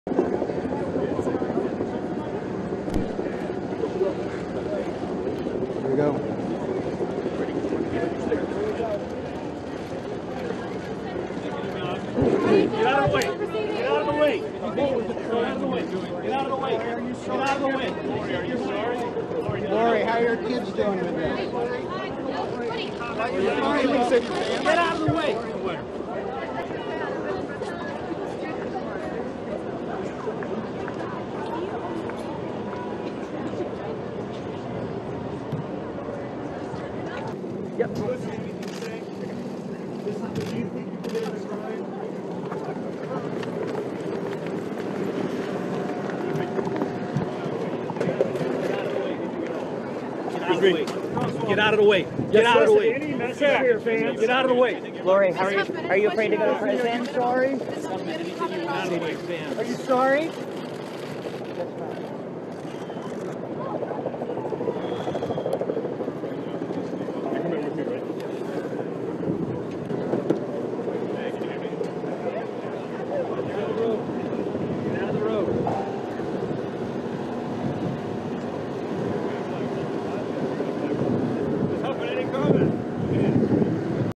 Video: Lori Loughlin, Felicity Huffman leave federal court
Actresses Lori Loughlin and Felicity Huffman faced court appearances Wednesday on charges they took part in the college bribery scandal that has ensnared dozens of wealthy parents.